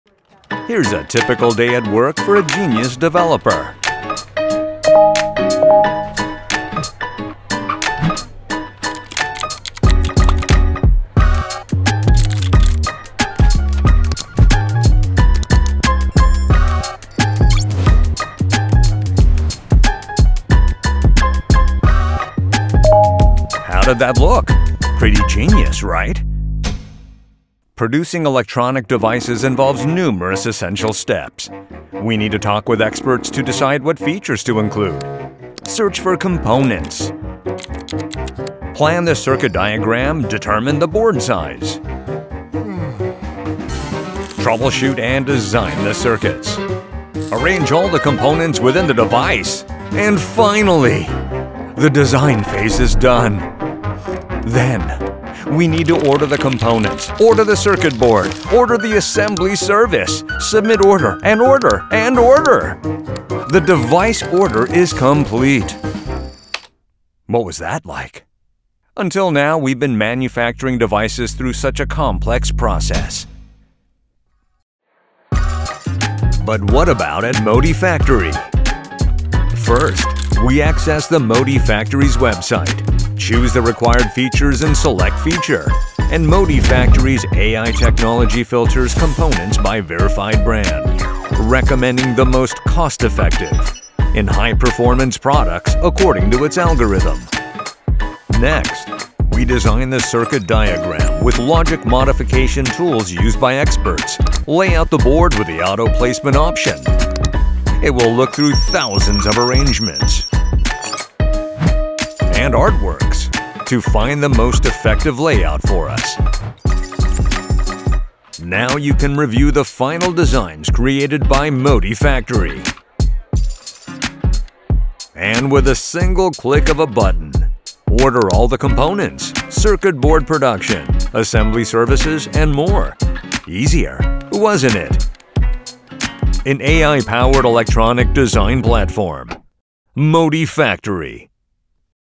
MODI Factory | AI-Driven Circuit Design Innovation (Full ver.)
MODI Factory is an integrated circuit development solution that utilizes AI technology to support fast and easy electronic circuit design, from design to manufacturing.

With its intuitive design UX and one-stop ordering feature, it significantly reduces development time and costs.

This solution is the ideal choice for developers aiming to enhance design quality and productivity, as well as for managers looking to efficiently manage team resources.

Curious to learn more about MODI Factory?